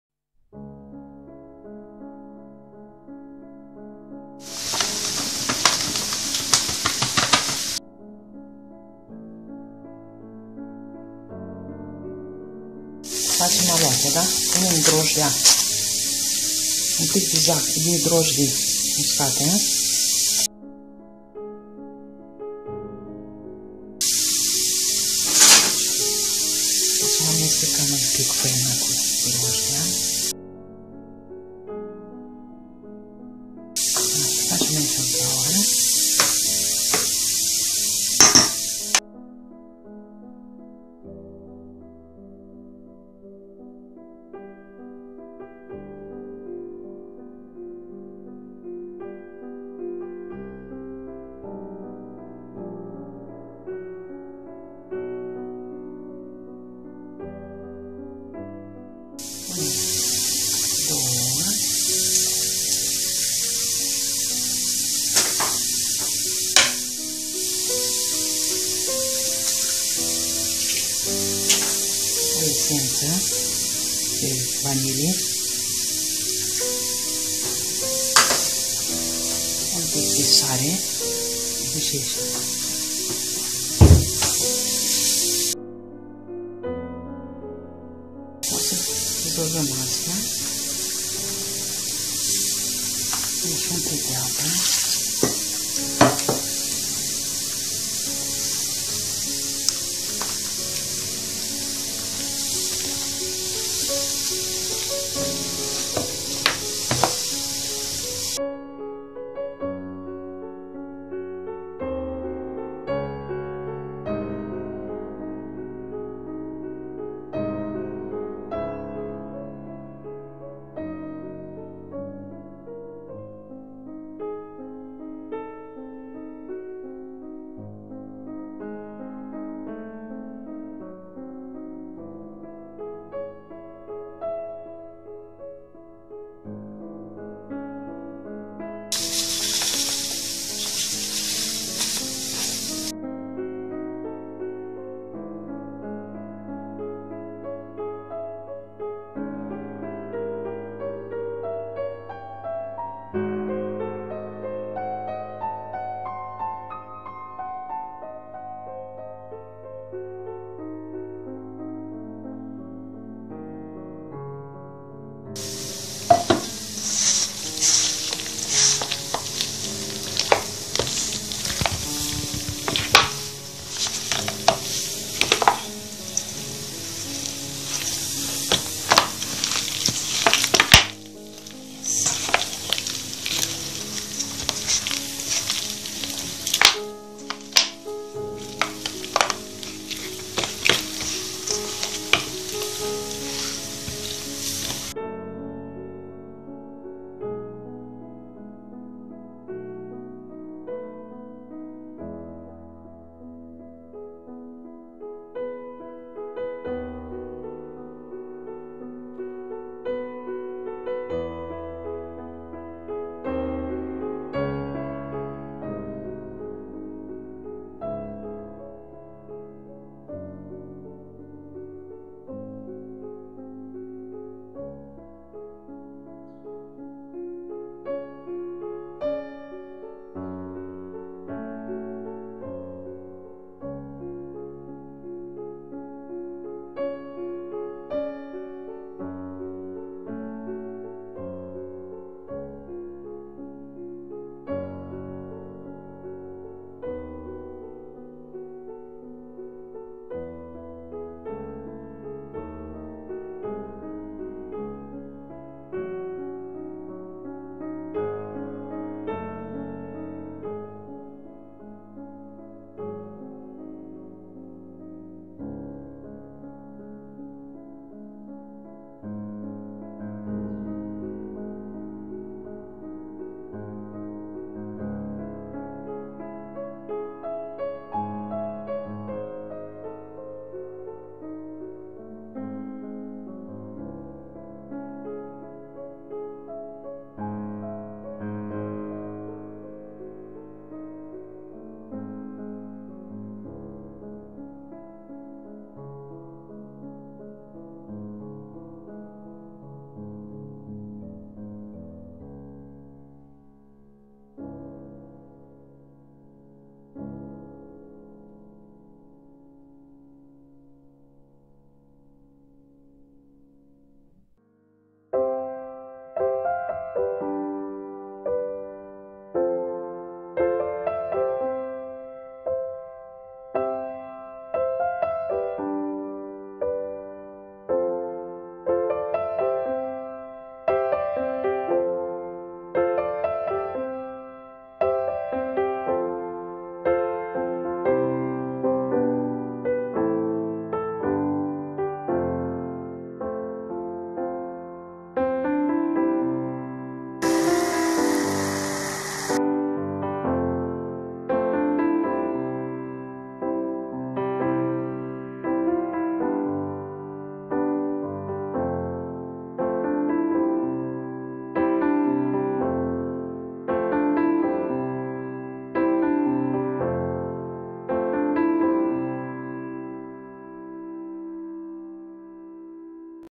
Langosi simple
Langosi simple - reteta traditionala de la mama
Se pot servi cu smantana si cascaval ras pe deasupra